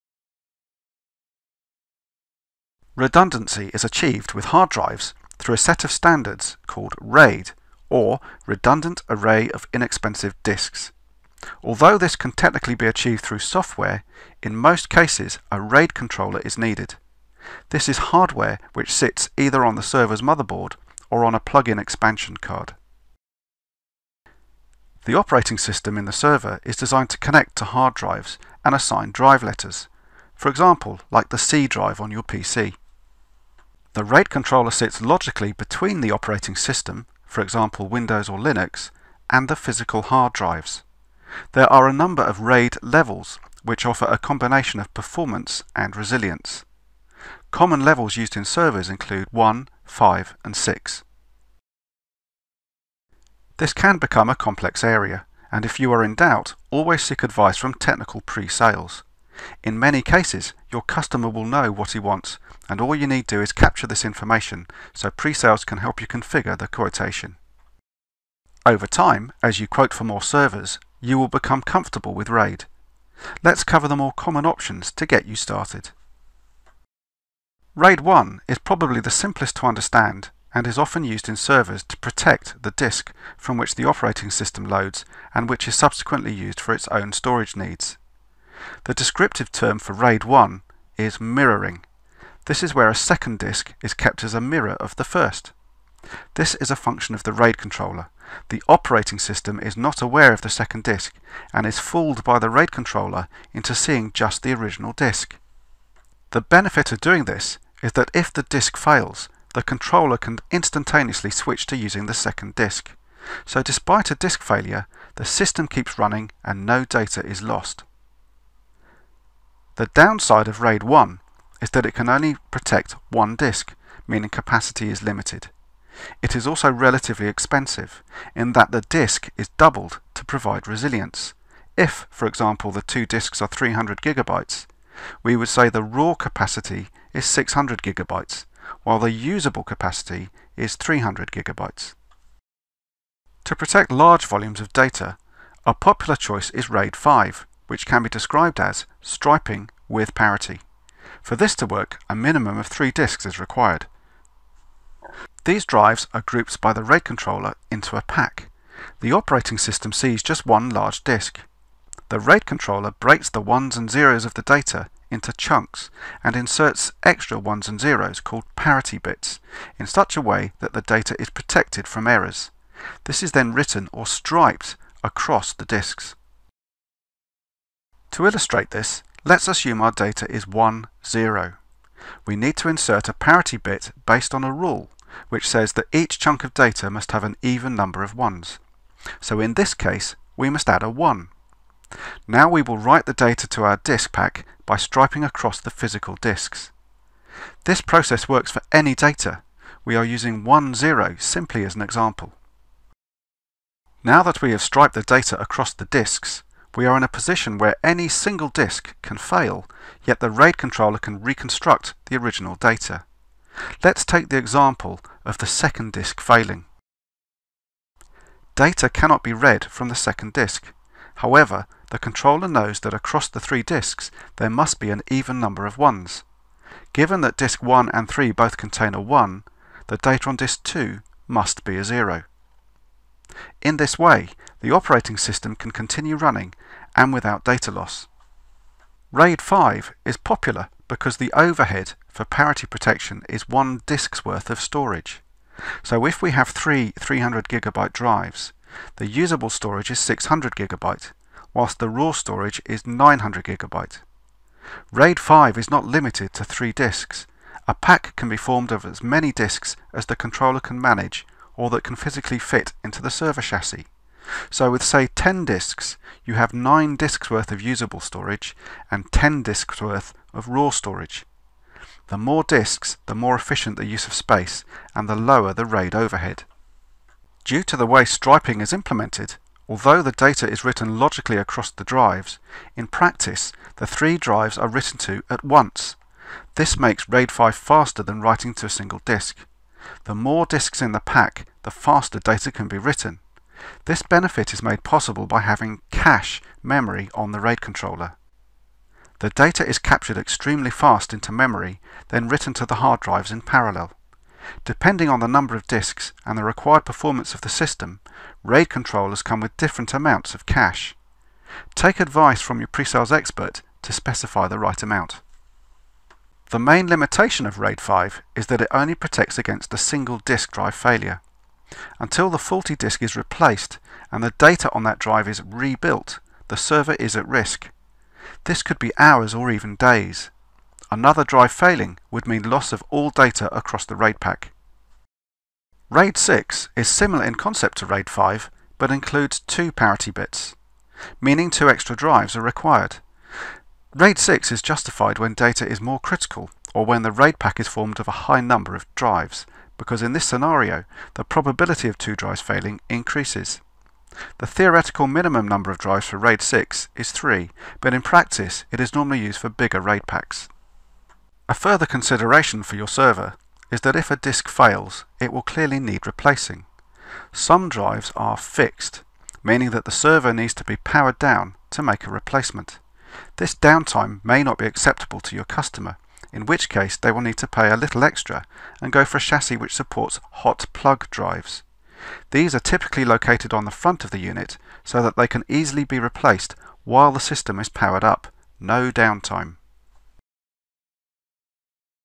What is RAID
Use RAID for performance and/ or data protection in your PC, Server or enterprise storage system. Covers RAID 1,5 and 6.